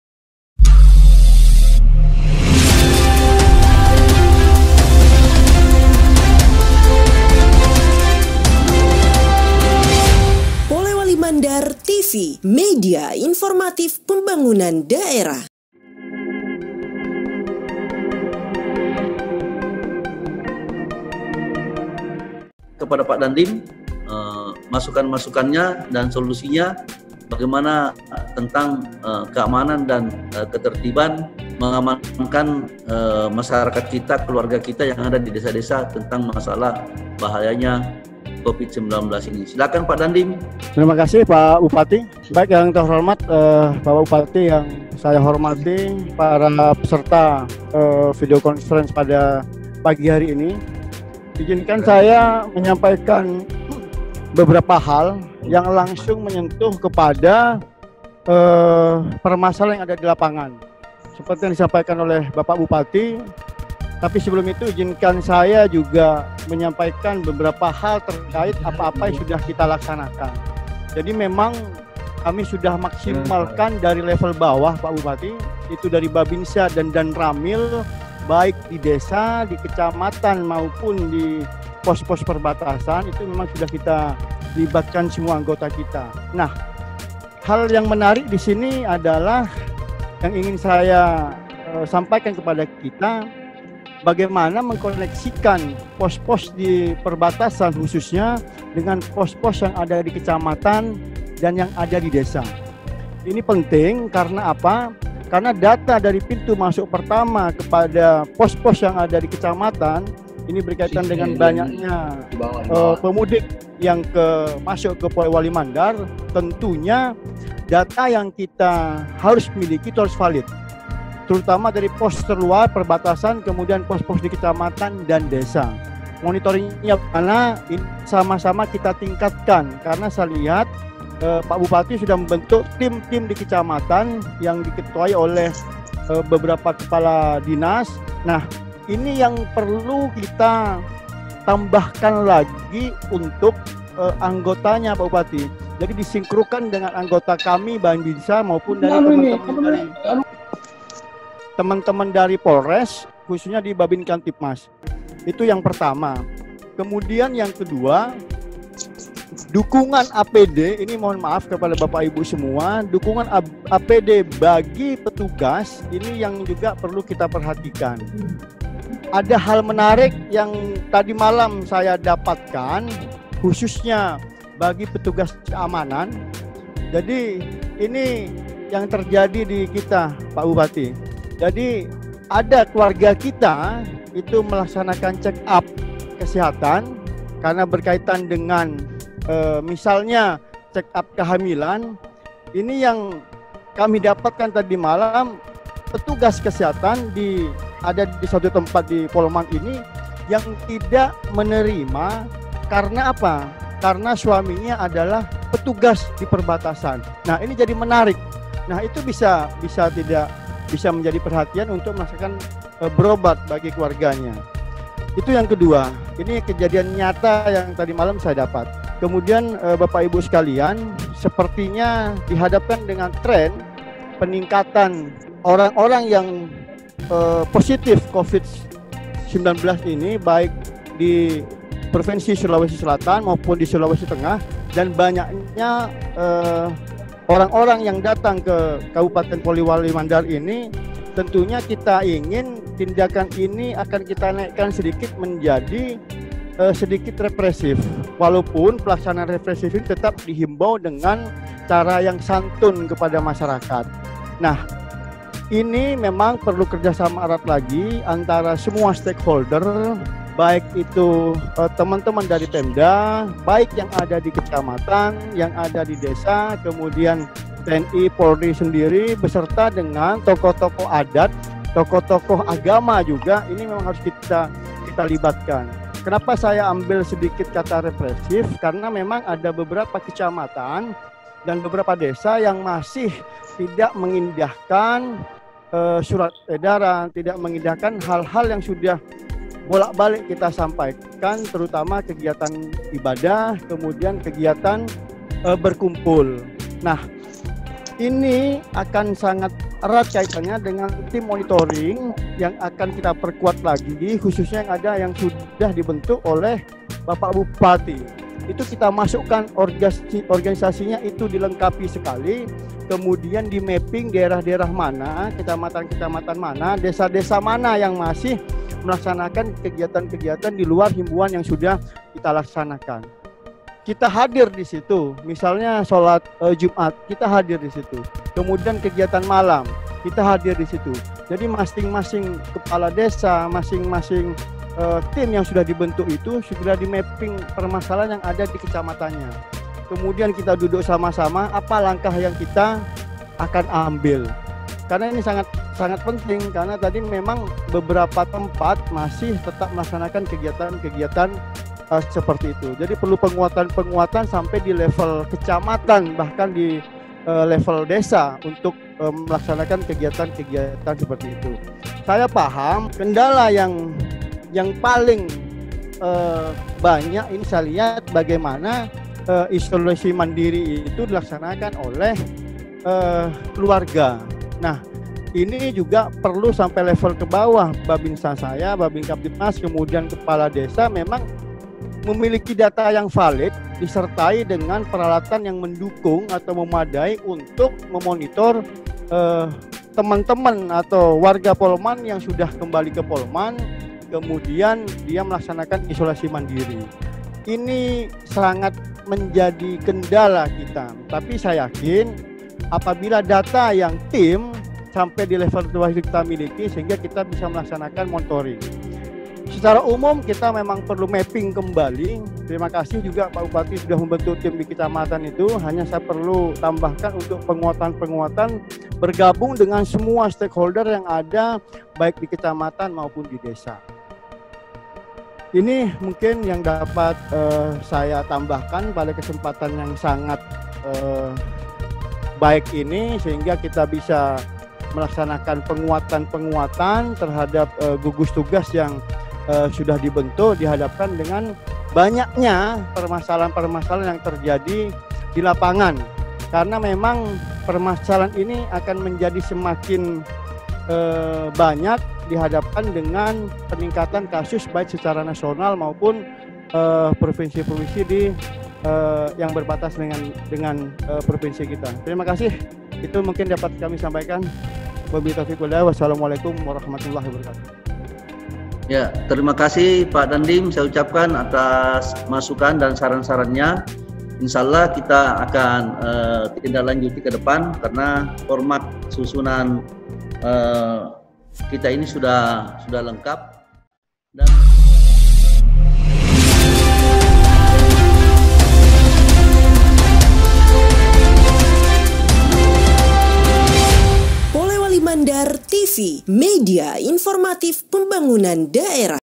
RAPAT KOORDINASI BUPATI DAN JAJARAN PIMPINAN KABUPATEN POLEWALI MANDAR (Bupati & Dandim1402 Polmas)
Agar Channel Youtube ini bisa berkembang... dan bersama kita share informasi ini, agar bermanfaat bagi khalayak demi membantu mengatasi virus Covid-19 atau CORONA.